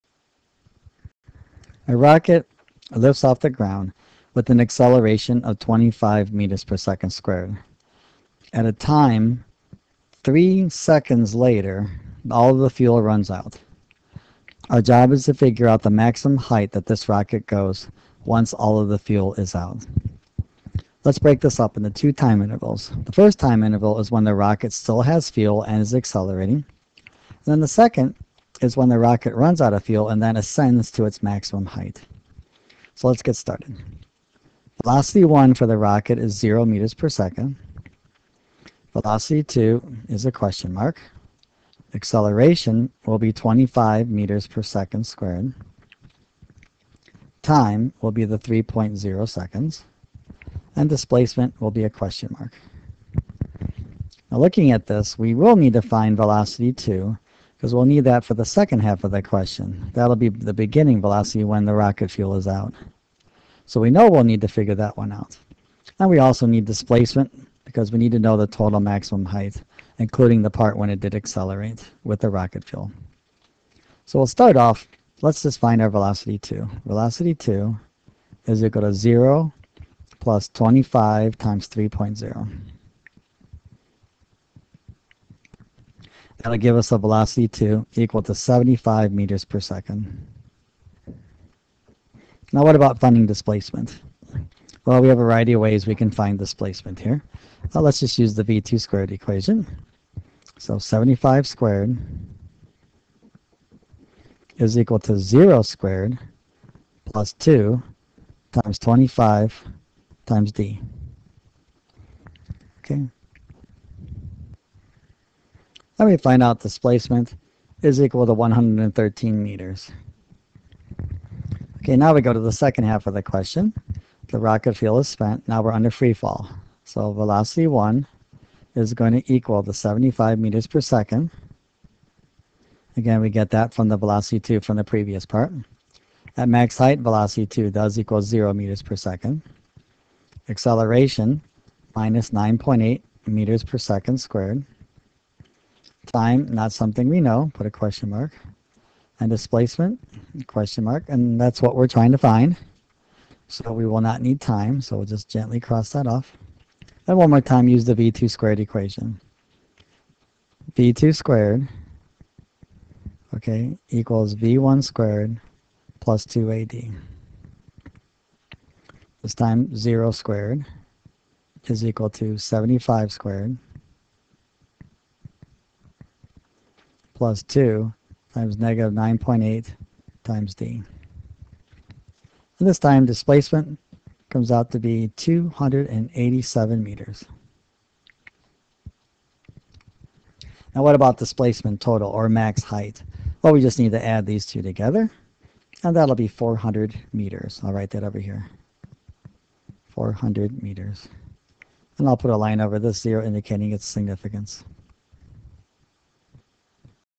Rocket Maximum Height
The maximum height for a rocket is calculated after it runs out of fuel.  The question is broken into two time intervals, and simple kinematic equations are used to determine the maximum height,